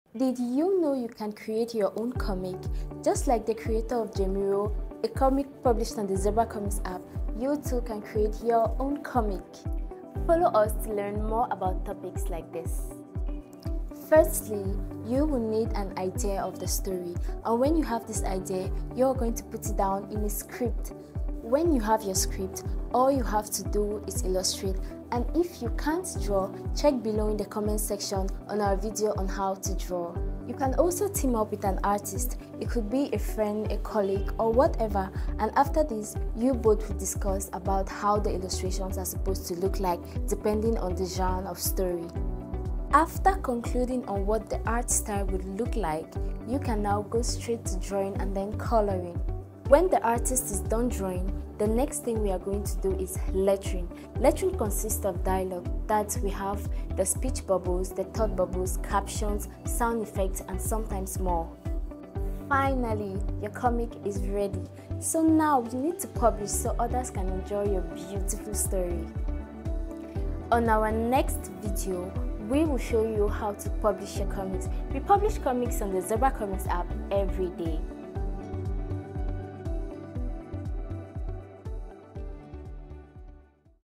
DID YOU KNOW YOU CAN CREATE YOUR OWN COMIC?
The creation process for many can be cumbersome and a little difficult for people who think they have no talent. Well, we believe that with patience, commitment and discipline, you can do a pretty decent job. Check out the video below and discover the different steps in the comic book création process.